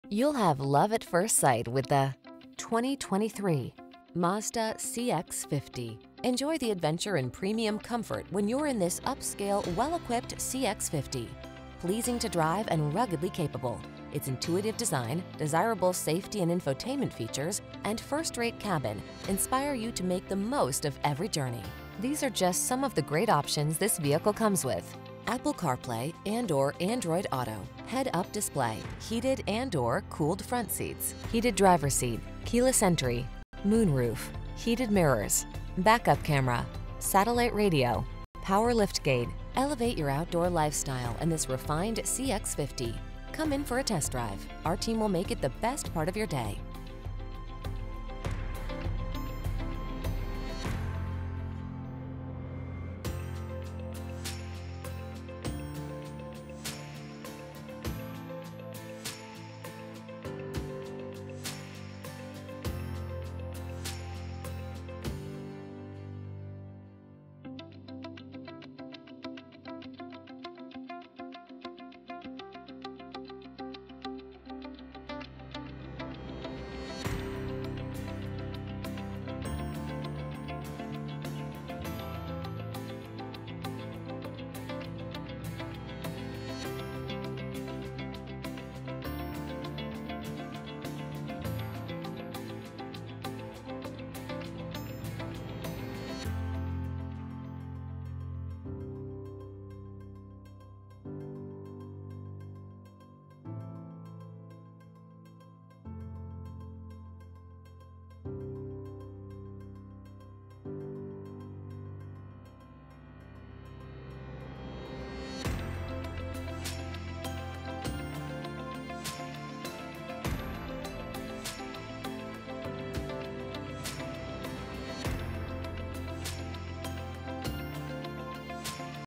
2023 Mazda CX-50 Arvin, Oildale, Shafter, Wasco, Bakersfield, CA 71011
Ingot Blue Mica New 2023 Mazda CX-50 2.5 S Premium Plus Package available in Bakersfield, CA at  Bakersfield Mazda. Servicing the Arvin, Oildale, Shafter, Wasco, CA area.
2023 Mazda CX-50 2.5 S Premium Plus Package - Stock#: 71011

Bakersfield Mazda
2900 Cattle Dr